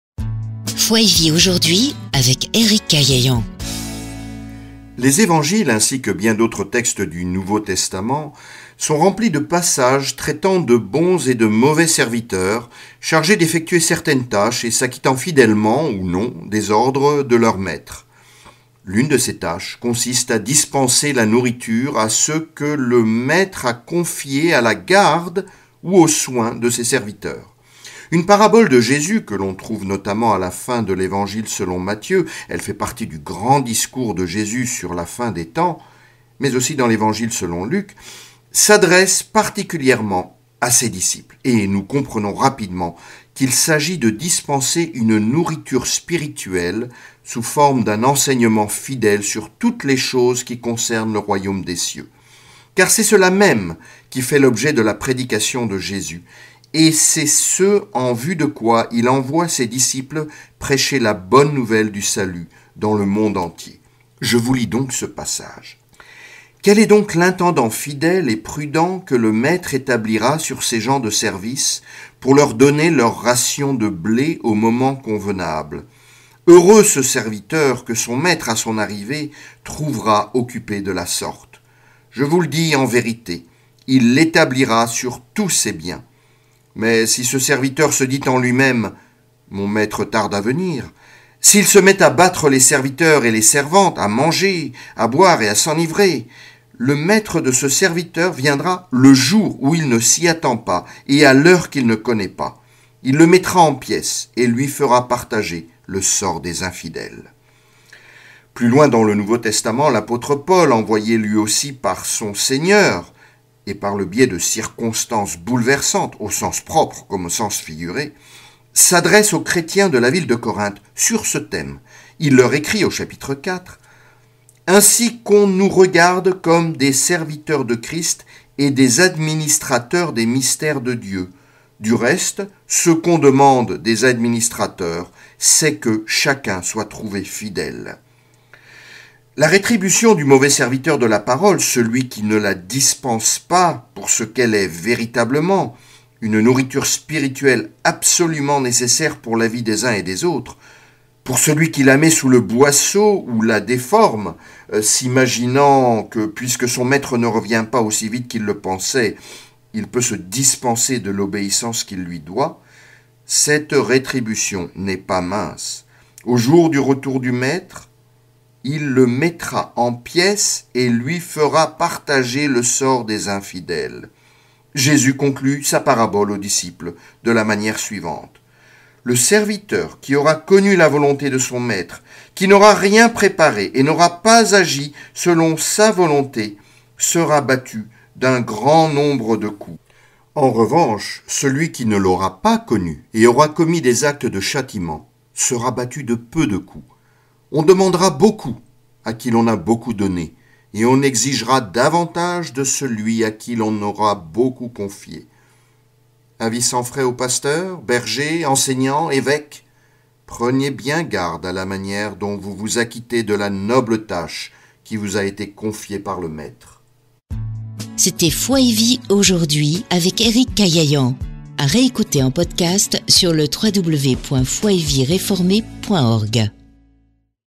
DISPENSER LA NOURRITURE
Dispenser une nourriture spirituelle: c'est le mandat des serviteurs envoyés par le Maître, et ils devront lui rendre des comptes de la manière dont ils se seront acquittés de cette noble tâche